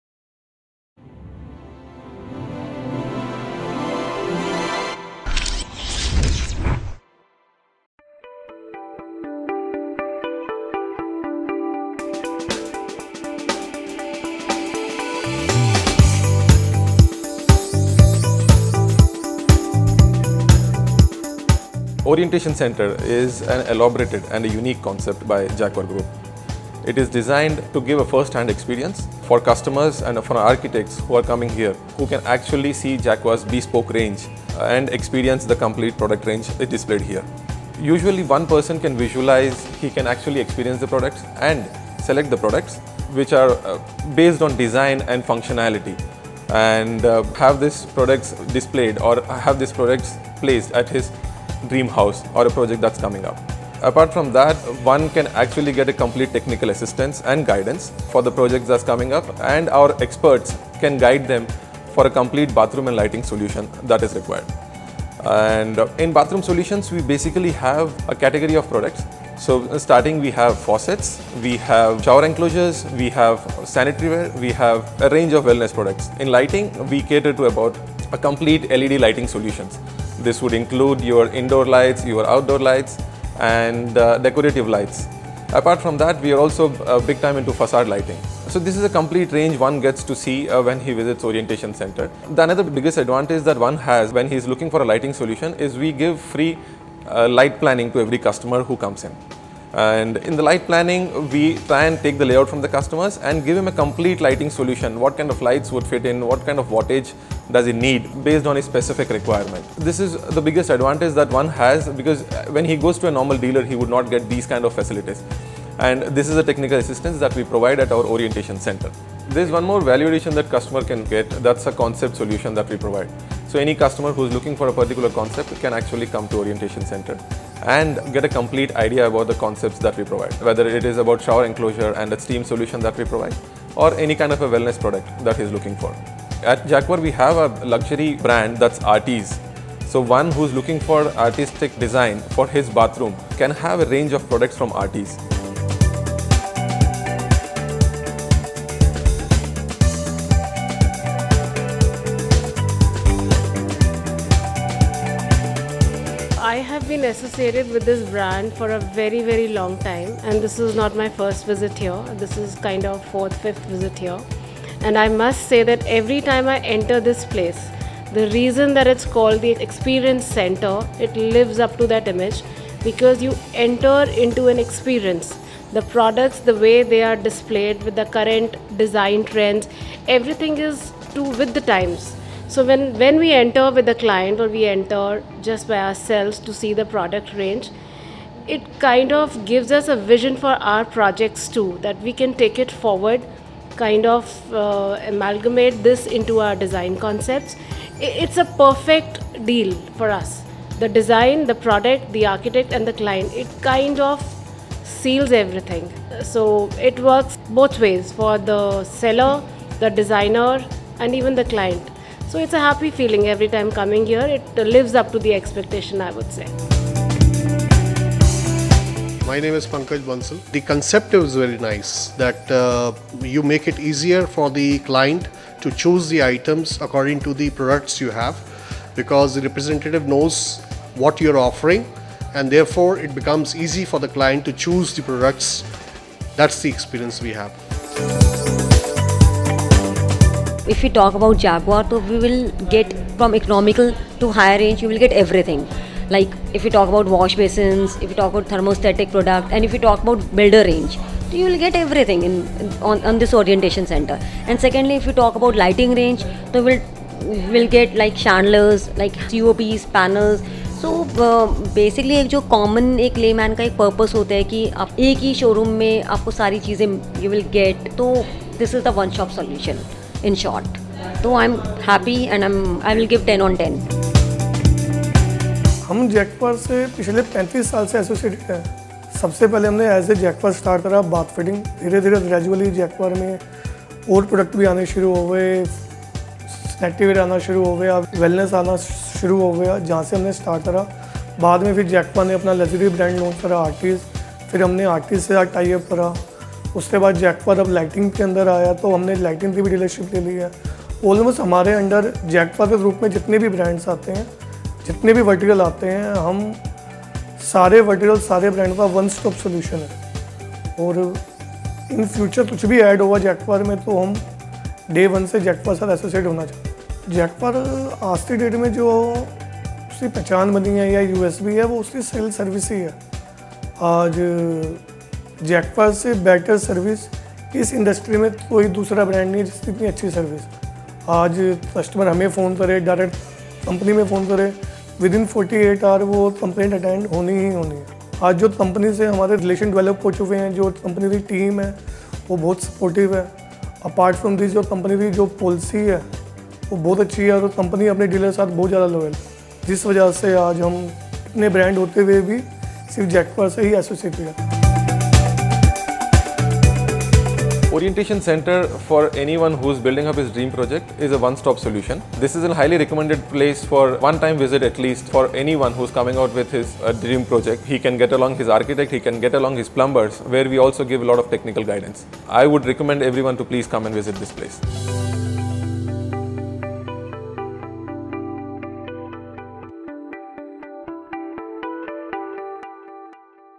Jaquar - The Best Bathroom Design Experience | Orientation Centre, Defence Colony
Jaquar Orientation Centres have been designed to provide a first-hand experience of Jaquar’s bespoke range of complete bathroom and lighting solutions; featuring complete bathroom products like faucets, showers, sanitaryware, shower enclosures, etc. and complete LED lighting solutions for residential, commercial, and outdoor.

At Jaquar Orientation Centres, you can experience, enlighten and interact with our specialists. Dedicated to the homeowners, industry experts, architects, interior designers, and decor aficionados to dwell into the world of Jaquar and explore the nuances of designer bathroom & lighting products. It's a must-visit place to experience the perfect blend of modern aesthetic & innovative design.

Produced By:
Building Material Reporter a product by ‘Business Mantra Reporter Pvt. Ltd’

Vanity lights review
Orientation centers
Led façade light
Jaquar chandelier